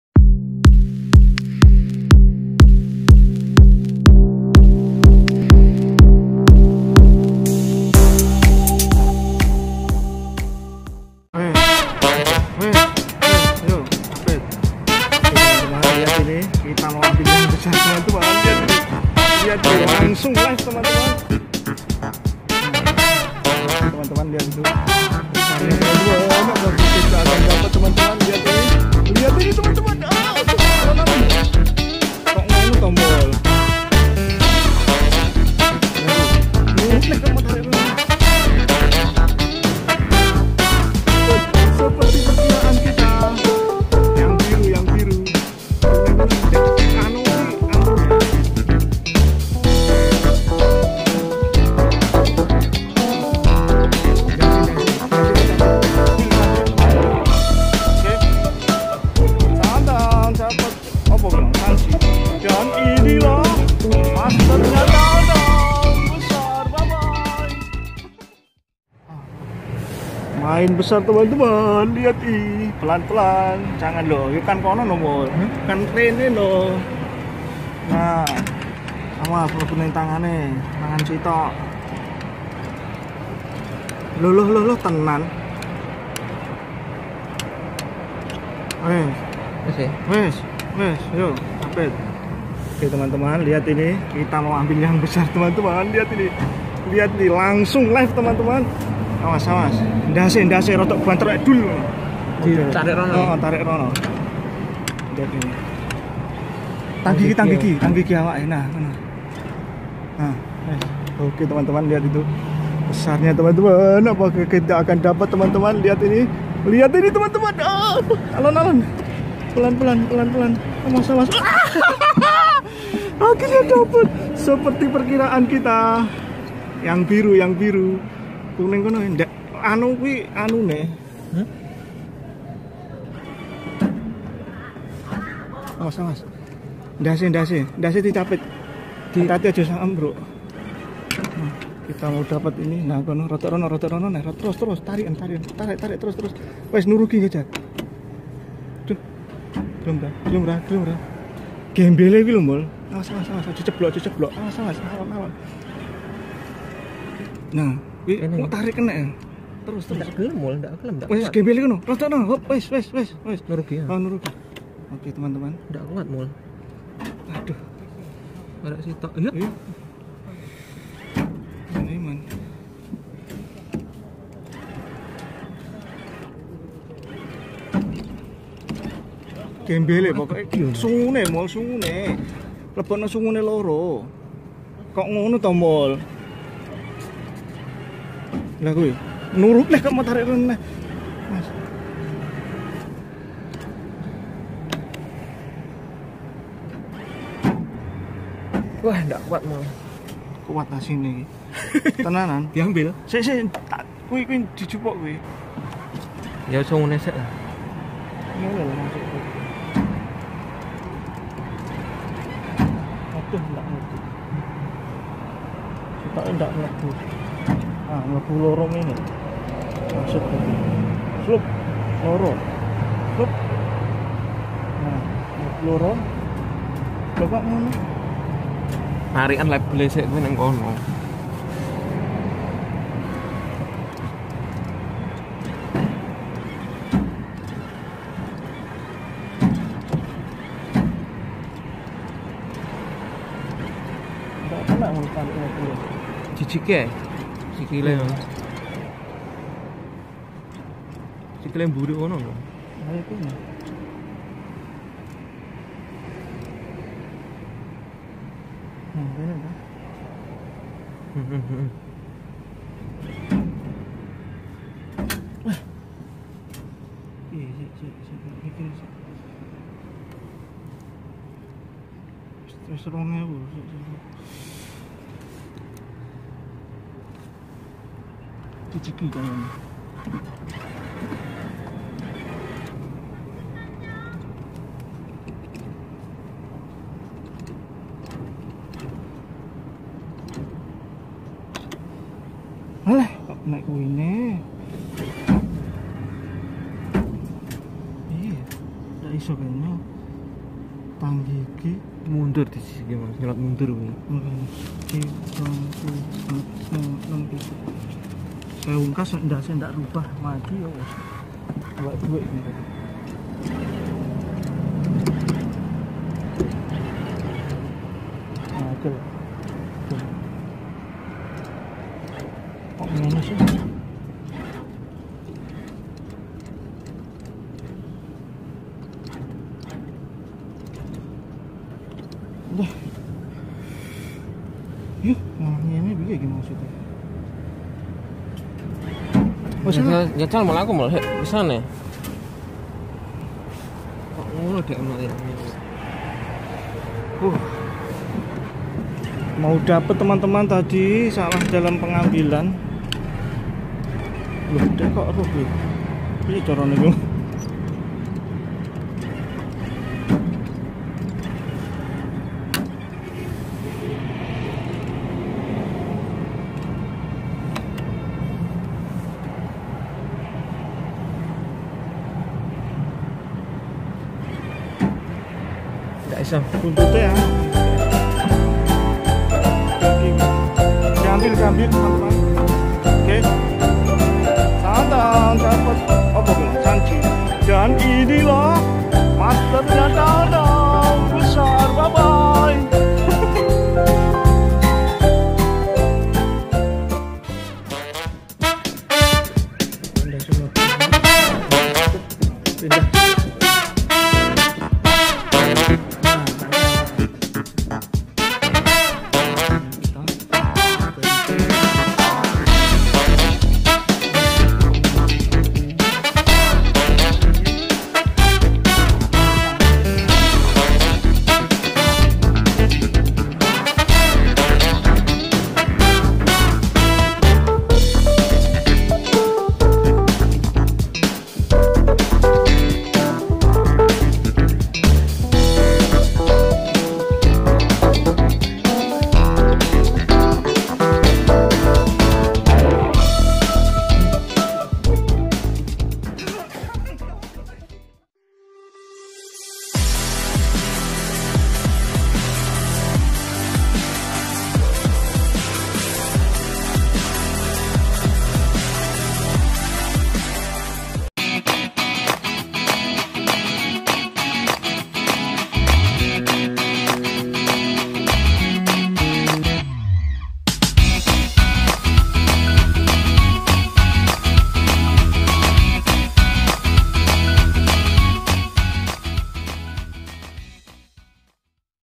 pokicrane ( CAPIT BONEKA ) LAGI-LAGI DAPET BONEKA BESAR
sesuai judul kali ini dapetin boneka besar dengan mudah hanya beberapa kali capit kami sebenarnya udah dapetin bonekanya .dikarenakan kami juga incar boneka pikachu besar jadi kami agak keluar uang banyak, namun gpp yang penting hepiii. 😊😊😊